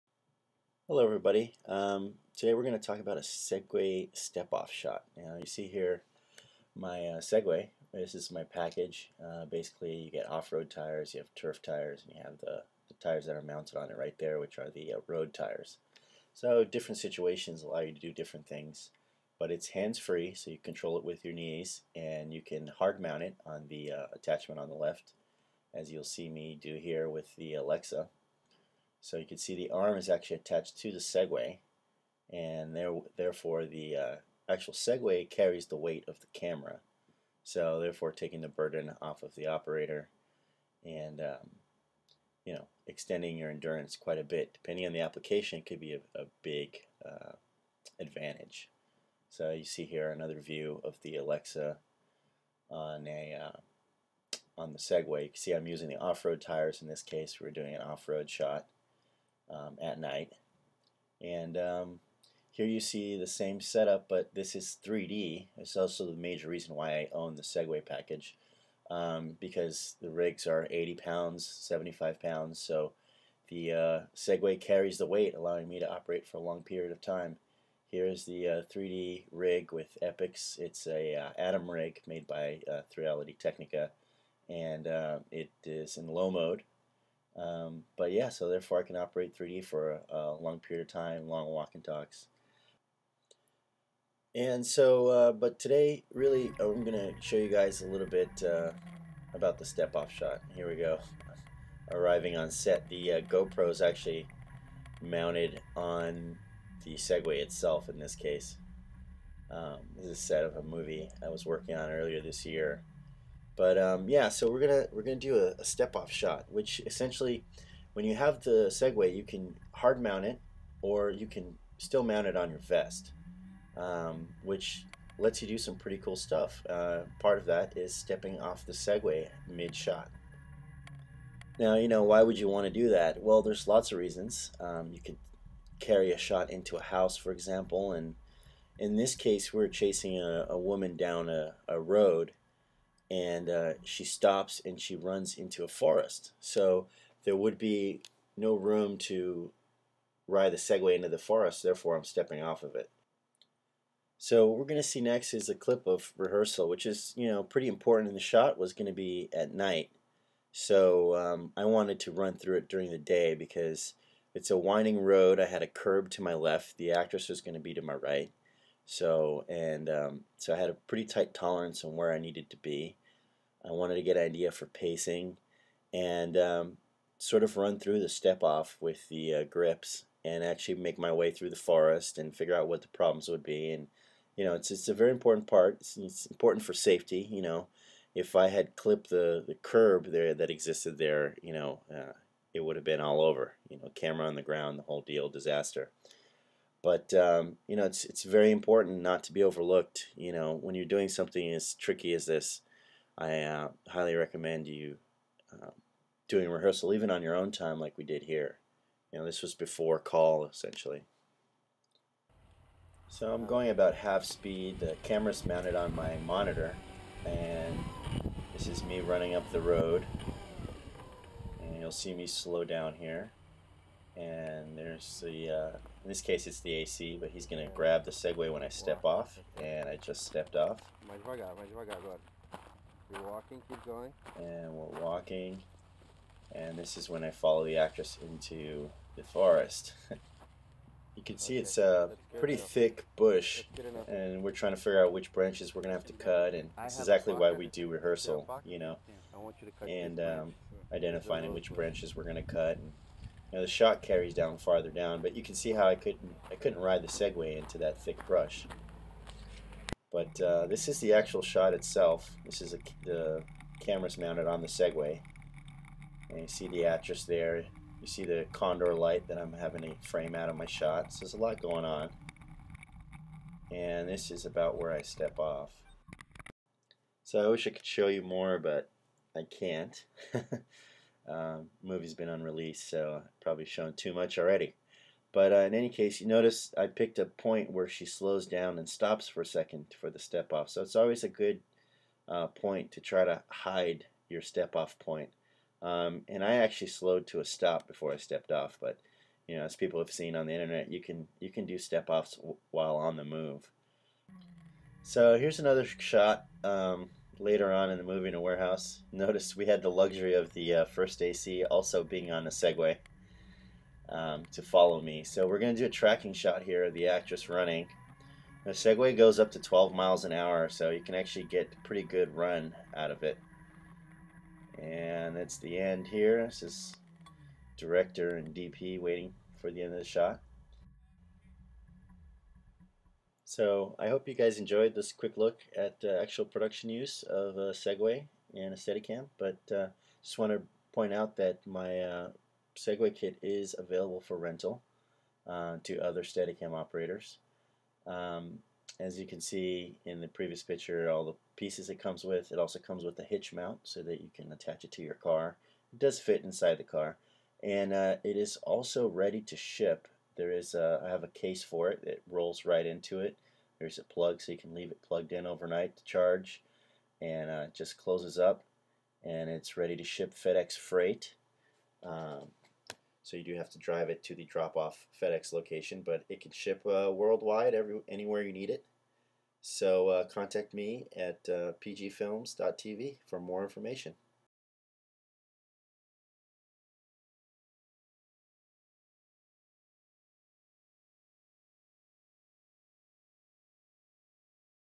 Anatomy of a Steadicam/Segway "step off" shot.
Take a look and the use of a handsfree segway behind the scenes on a major motion picture. Operator and owner, Pedro Guimaraes, SOC takes you for a ride you will not forget!

More info at:
PGFILMS.tv

and more articles

Steadi3d.com